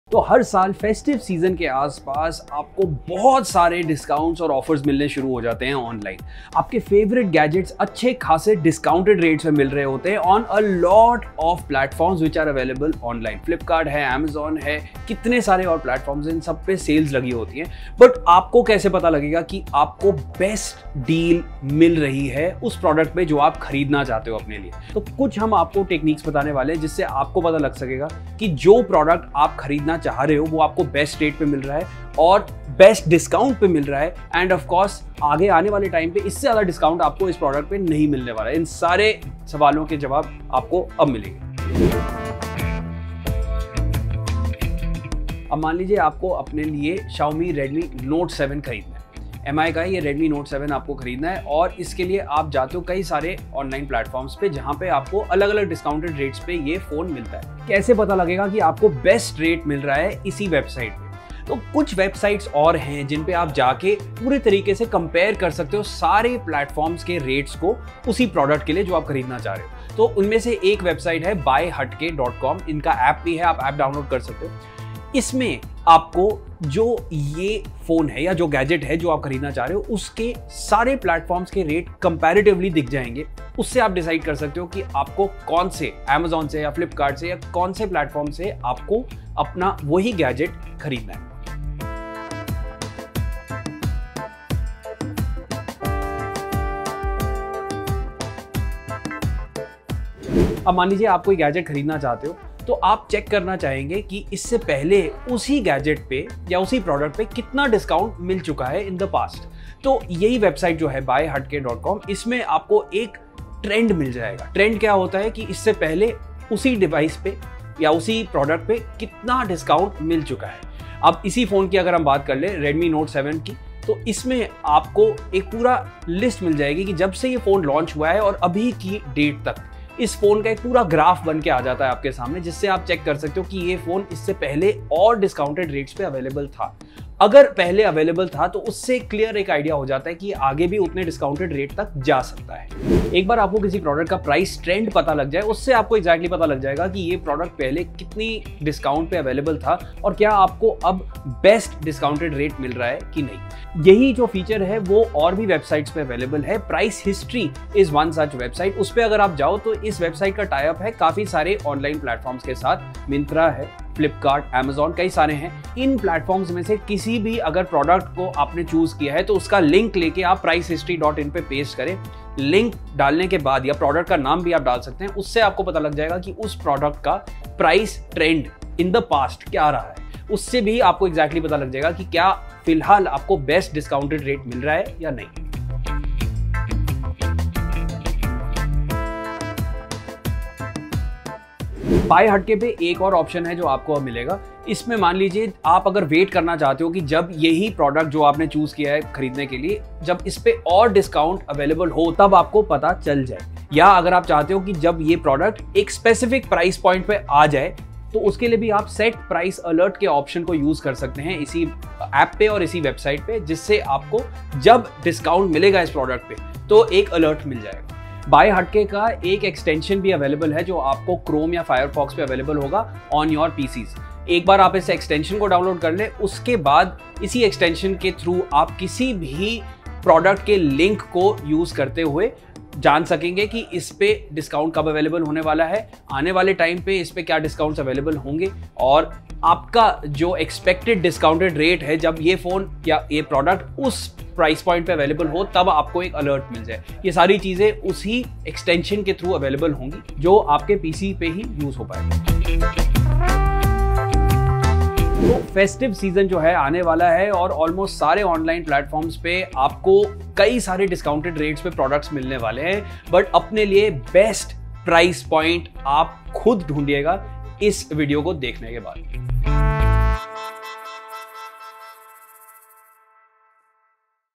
ऐसे होगी ऑनलाइन शॉपिंग पे ज़्यादा से ज़्यादा बचत | ONLINE SHOPPING TRICKS | Tech Tak
Here are some tricks to help you get the best discounts while shopping online. Get the best deals and reap maximum benefits from your online shopping experience.

Update to a better version of yourself, every month.
Also, get a chance to win a brand new Maruti Suzuki Ciaz!